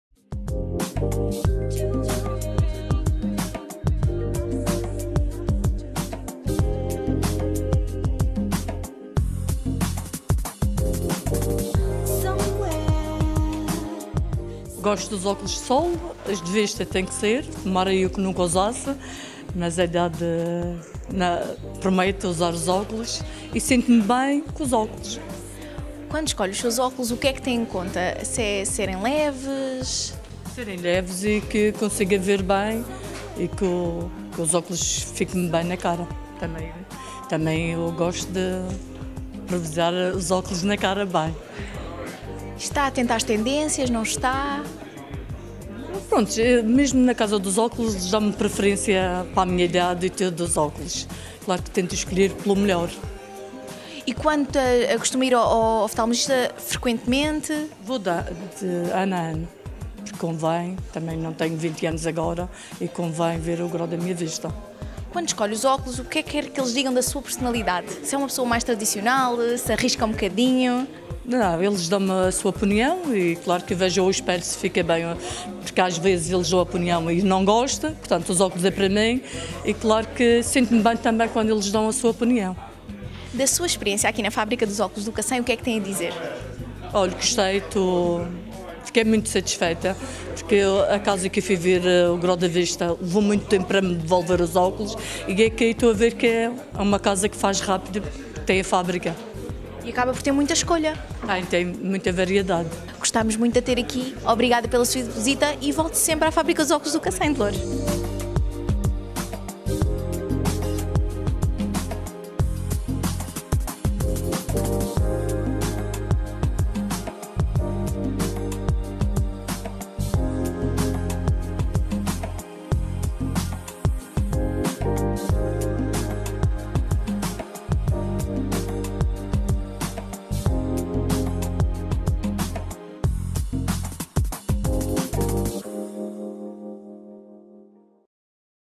Dolores Aveiro
A mãe do Cristiano Ronaldo ouviu falar da Fábrica de Óculos do Cacém e decidiu vir fazer-nos uma visita. Ouça o seu testemunho e fique a saber porque somos a marca preferida dos portugueses.